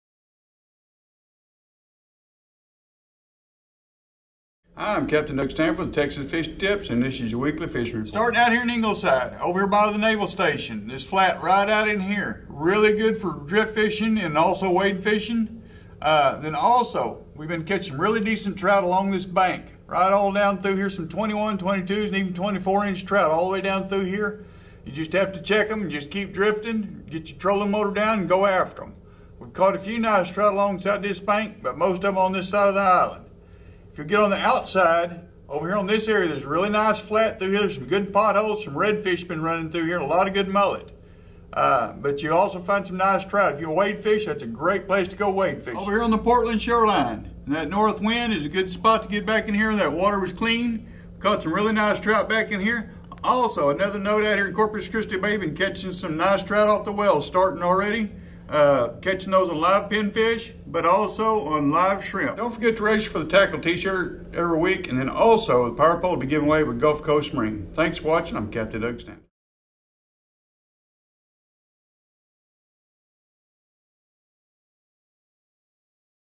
Texas Fishing Tips Fishing Report April 25 2013 Corpus Christi Bay Area
Texas Fishing Tips Fishing Report April 25 2013 Corpus Christi Bay Area With Capt. Doug Stanford